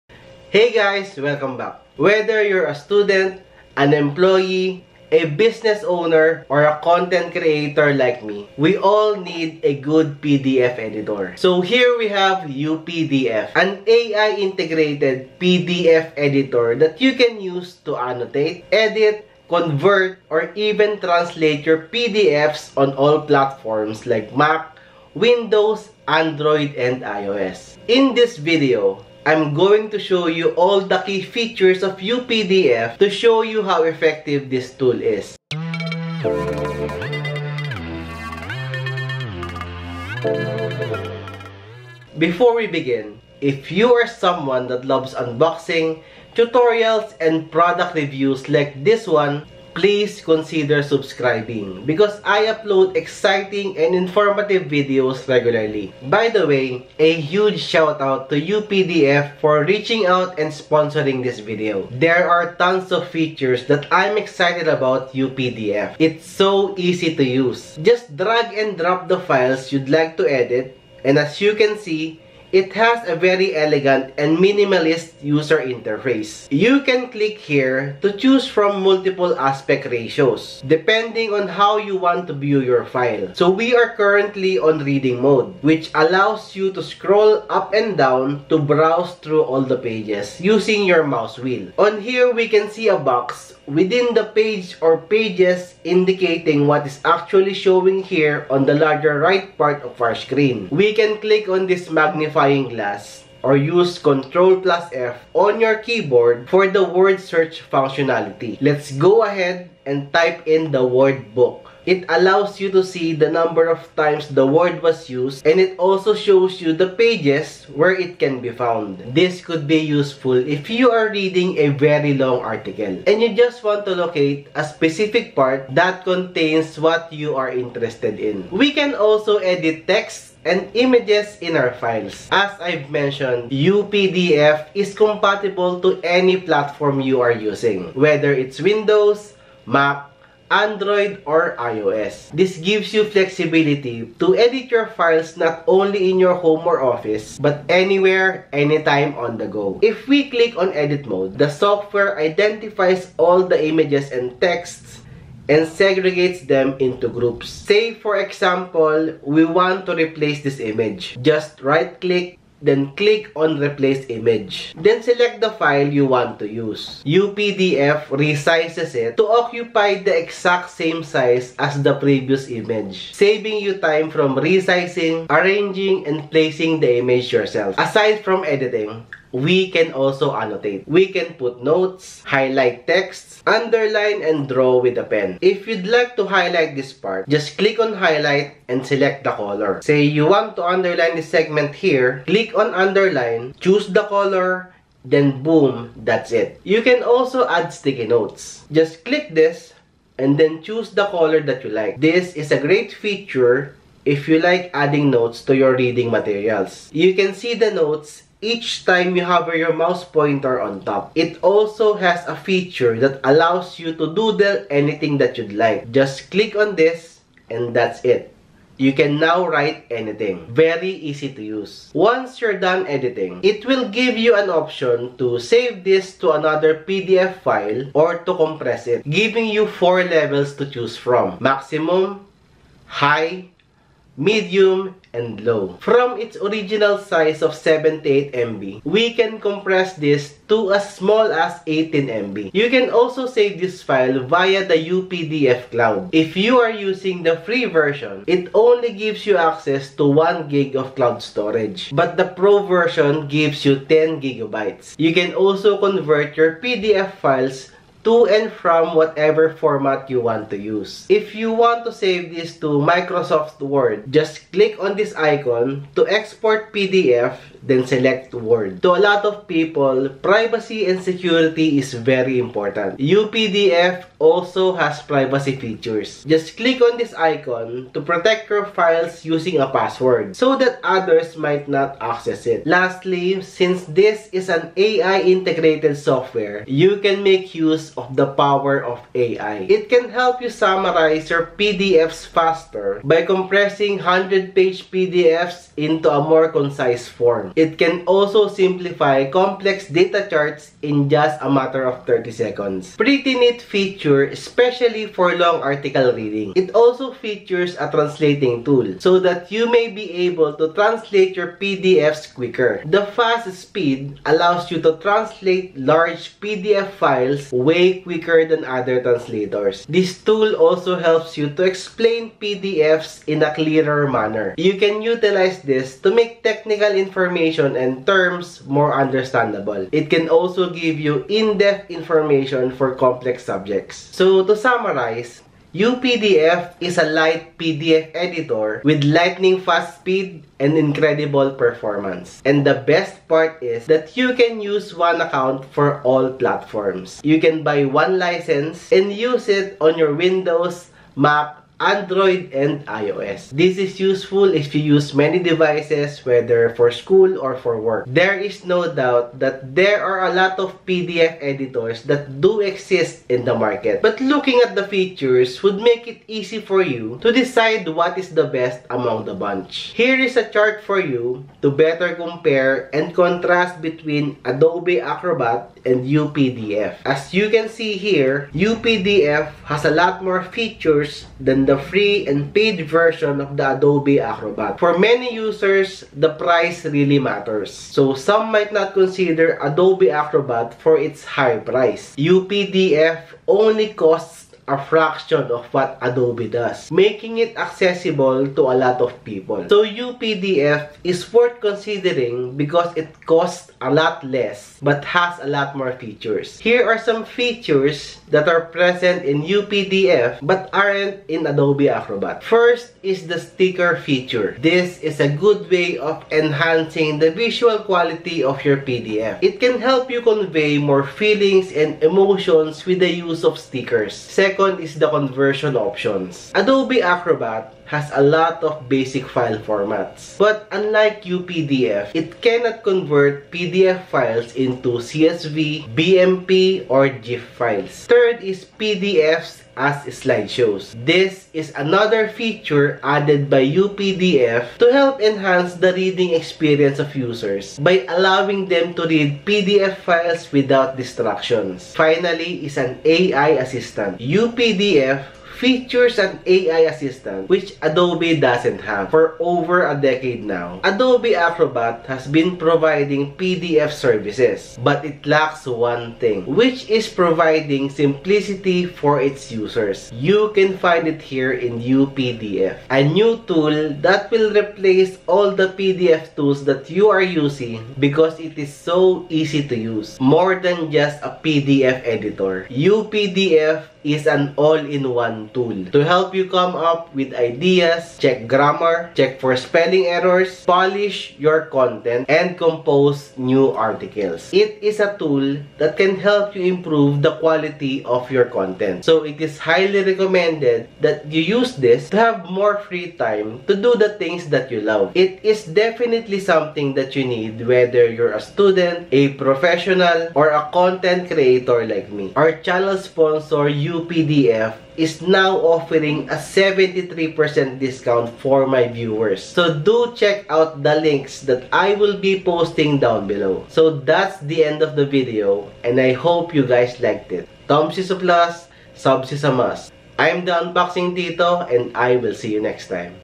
UPDF Review | The Best PDF Editor Alternative to Adobe!
Would like to find a perfect professional PDF editor? An alternative to Adobe Acrobat? Here is the video review for UPDF Editor!
UPDF is the best AI-integrated PDF editor to annotate, edit, convert, OCR, protect, fill, sign, summarize, translate, and explain PDFs.  One License Can be used on All Platforms to Edit and Sync PDFs!